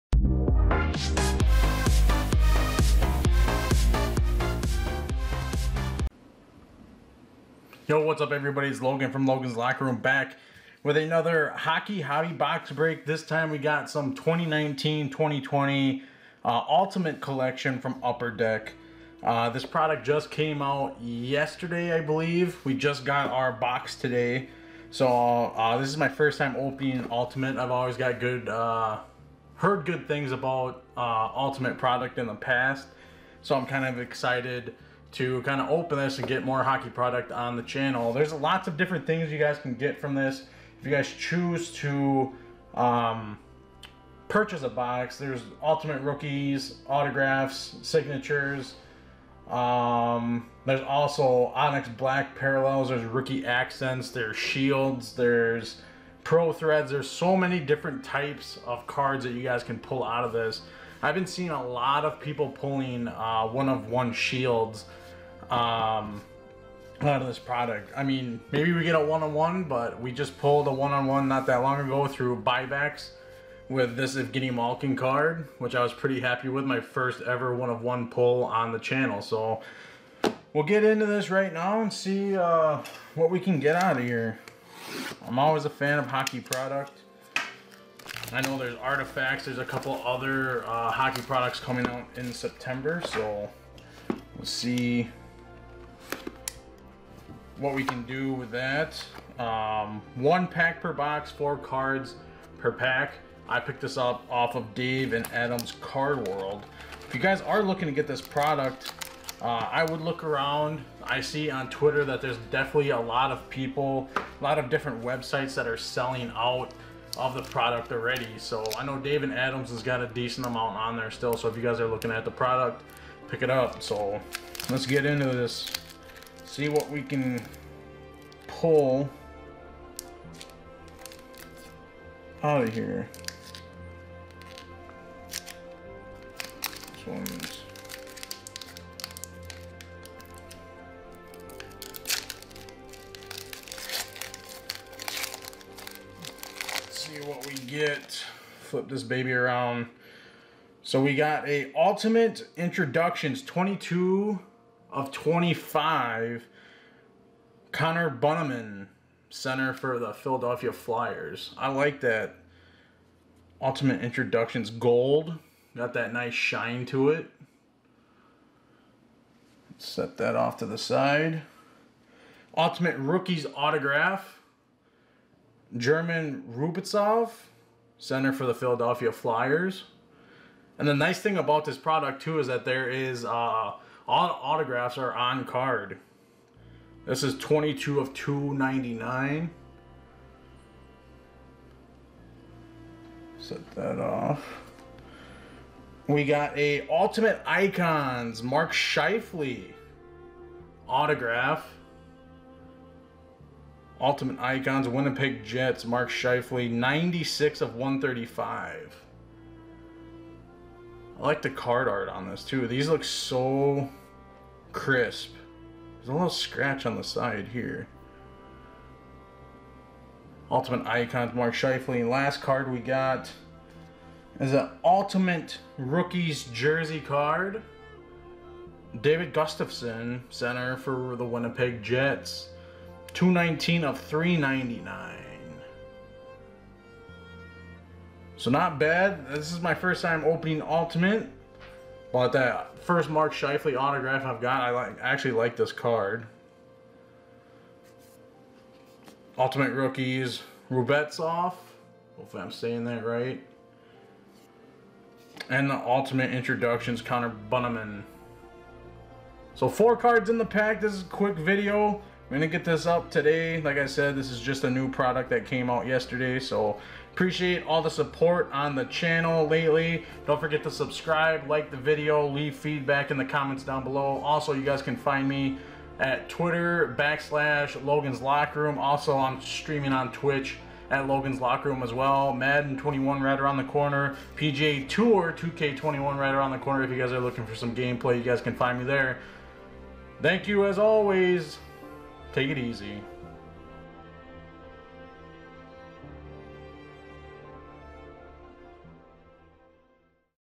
2019/20 Upper Deck Ultimate Collection Hobby Box Break
We open one hobby box of 2019/20 Upper Deck Ultimate Collection Hockey!